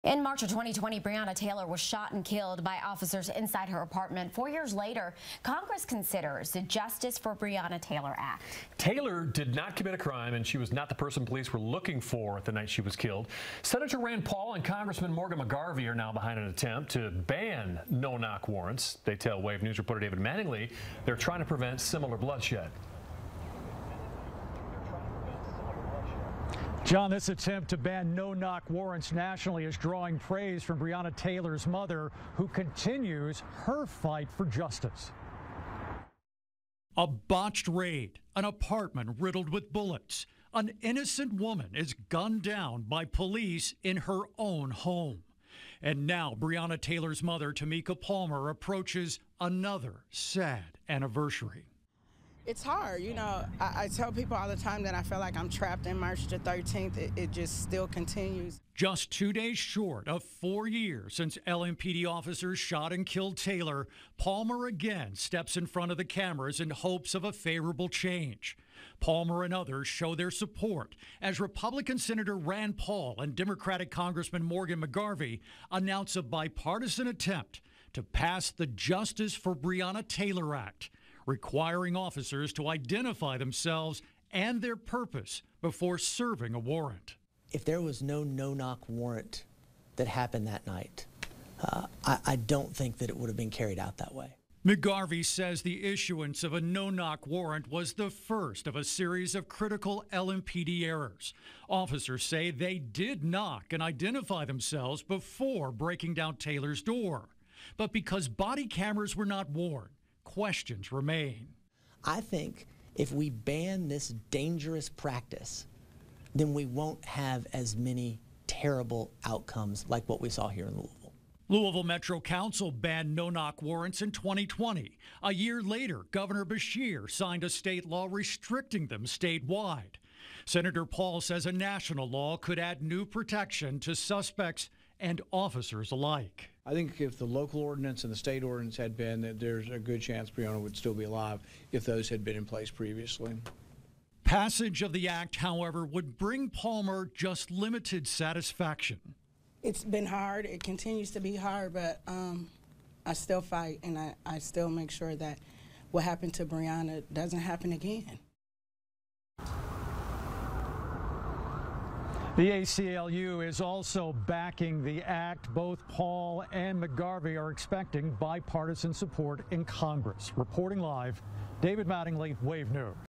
Morgan McGarvey, Rand Paul introduce ‘Justice for Breonna Taylor Act’ in Congress
Four years after she was shot and killed by LMPD officers, Congress is considering the “Justice for Breonna Taylor Act."